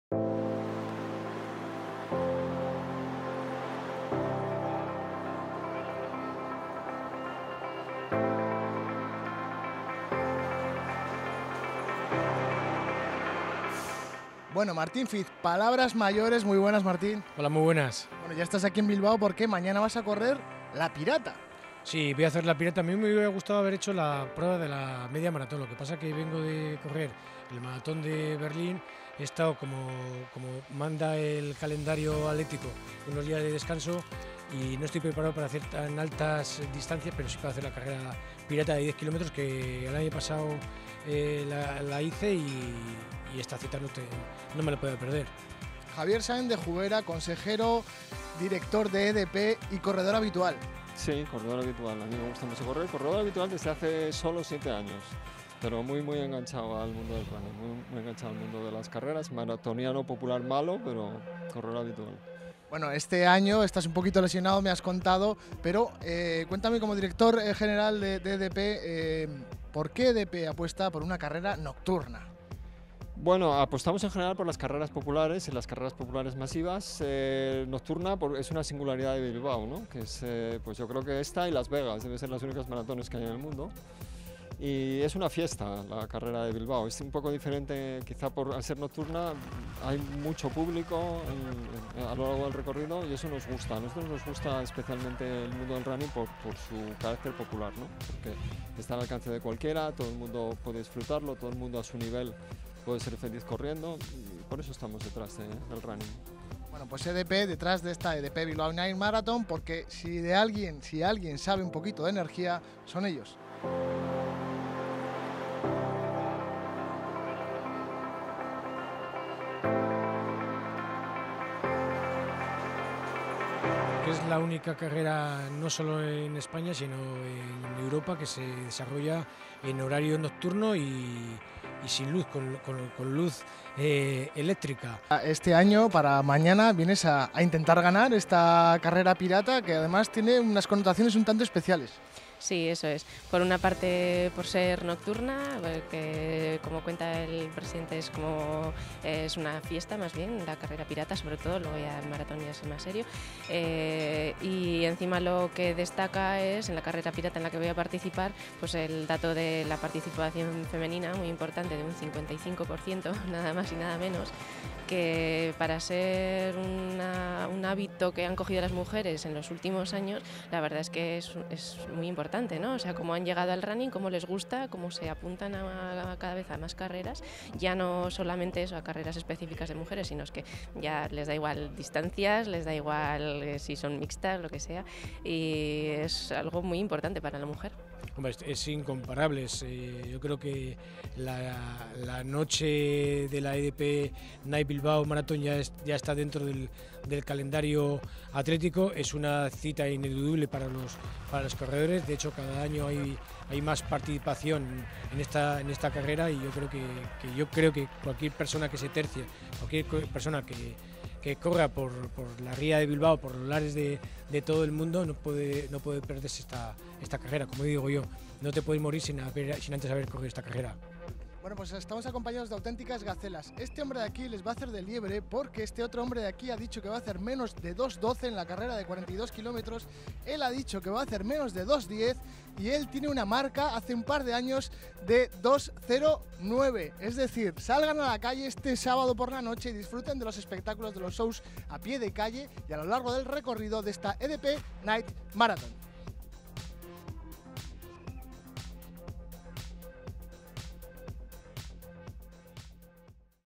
ATLETAS TOP EN LA EDP BILBAO NIGHT MARATHON 2016 MARTÍN FIZ ELENA LOYO
Entre los más de 11.000 corredores, que afrontarán el desafío de correr las pruebas de 42k, 21k y 10k, sobresalen las figuras de Martín Fiz, Elena Loyo, Camilo Santiago, Kaoutar Boulaid -dispuesta a atacar el récord femenino de la prueba- y un gran elenco de atletas africanos
Martín Fiz, todo un ilustre sobre el asfalto bilbaíno

Comencemos por Martín Fiz, campeón mundial (1995) y europeo (1994) de maratón, referencia universal de los 42km en la década de los 90… y en la presente, pues lleva cuatro triunfos (Nueva York, Tokio, Boston y Berlín) en su reto ‘Fiz Marathon Majors’, consistente en adjudicarse los ‘Six Majors’ (los seis maratones más prestigiosos del planeta) en la categoría Master 50. Le quedan Londres y Chicago, ambos en 2017, así que mientras recarga pilas nada mejor que alistarse en la versión pirata (10K) del EDP Bilbao Night Marathon. Al experimentado y veterano atleta también le acompañara su pupila Elena Loyo -plusmarquista alavesa con 33:20- la mejor fondista vasca de la actualidad, campeona de España de 10km y plusmarquista de Euskadi de media maratón (1:14:20). La corredora vitoriana suele contar por victorias sus incursiones en el asfalto, con victorias destacadas como el Medio Maratón de San Sebastián.

En los 21,097km defenderá título el riojano Camilo Santiago y en el maratón un elenco de consagrados keniatas tratará de darle un buen mordisco al crono. Al igual que Camilo en la media, Pharis Kimani tratará de reeditar laureles. Posee una marca personal de 2:12:03 y, a sus 32 años, está en plena madurez atlética, y en disposición de ofrecernos su mejor versión. Escoltas de lujo serán Stephen Kiplimo (acreditado en 2:10:03 y en busca del doblete vasco tras imponerse en el último Maratón de Vitoria), Eliljah Chelimo (mejor marquista de todos los participantes con 2:09:48 y primero este año en Chonquin, China) y Pius Kirop (otro sub 2:10 -2:09:55 en París- de talento incuestionable, pues en 2012 fue cuarto del mundo en medio maratón, donde posee una marca de 59:25).
Sin embargo, la gran mayoría de las miradas deberán posarse en la elegante figura de Kaoutar Boulaid, segunda clasificada en el Maratón de Sevilla, donde conquistó la marca mínima que le abrió las puertas a los Juegos Olímpicos de Río de Janeiro.

La atleta marroquí no tiene ante sí una empresa pequeña: batir el récord femenino de la modalidad más emblemática de esta fiesta deportiva llamada EDP Bilbao Night Marathon. El tope, 2:49:28, está en poder de Margaret Atodonyang desde 2010.

Todos ellos, y el resto de participantes tendrán su punto de encuentro en EXPORUNNING BILBAO, obligado paso para recoger su correspondiente dorsal. El gran punto de encuentro para corredores, expositores y personalidades destacadas del sector abrirá sus puertas en el Palacio Euskalduna, durante el viernes 21, en horario continuado de 15:30 a 20:30h y el sábado 22, también en horario ininterrumpido de 9:30 a 19:30h.